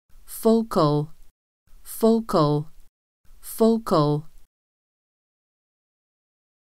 Pronounce Medical Words ― Focal
This video shows you how to say Focal.

How would you pronounce Focal?